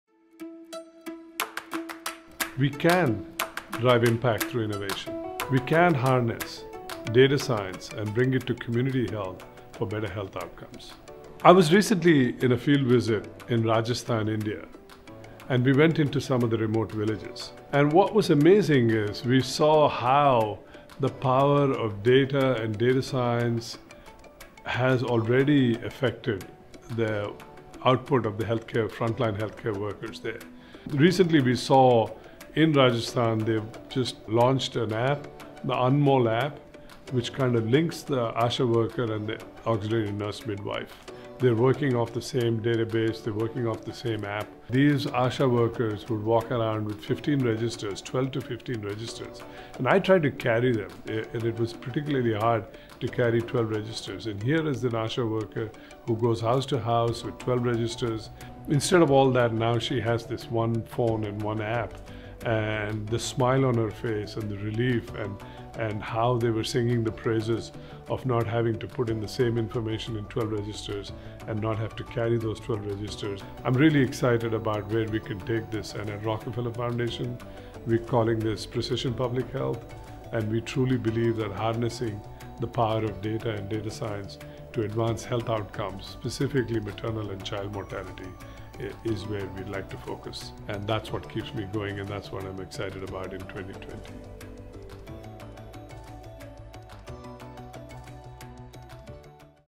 Dr. Naveen Rao on Precision Public Health | Creating ImpactThroughInnovation
Three weeks into 2020, our team reflects on how we can continue to create ImpactThroughInnovation. For Dr. Naveen Rao, Senior Vice President for our Health Initiative, it’s about harnessing data & datascience for better health outcomes through Precision Public Health.